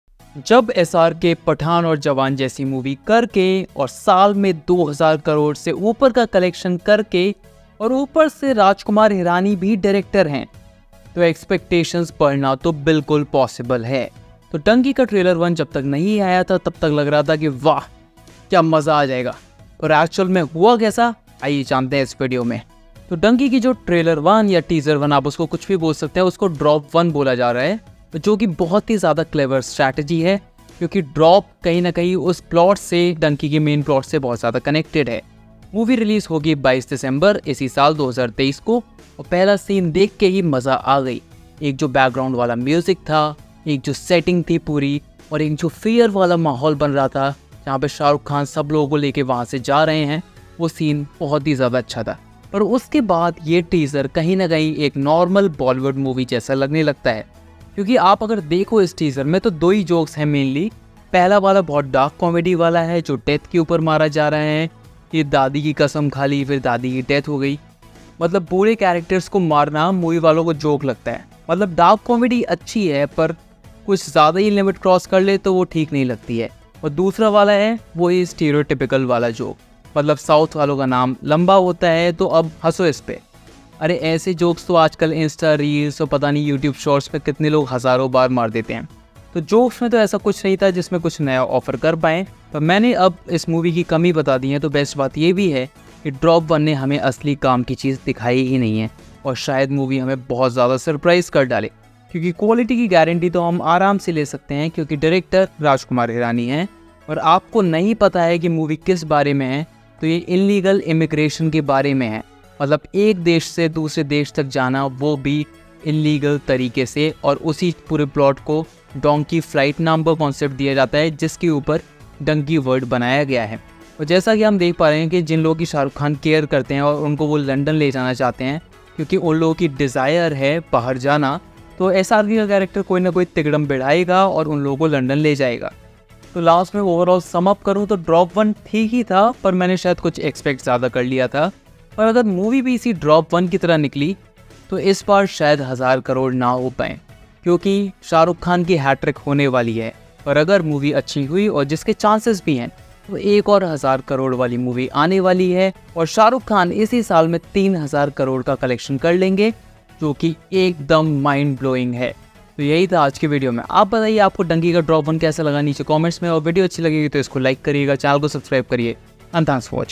Dunki Drop 1 Review | Shah Rukh Khan ki 2023 ka ek aur dhamaaka
Let's do the teaser review of Dunki starting Shah Rukh Khan, Tapsee Pannu and Vicky Kaushal. The film is directed by Rajkumar Hirani.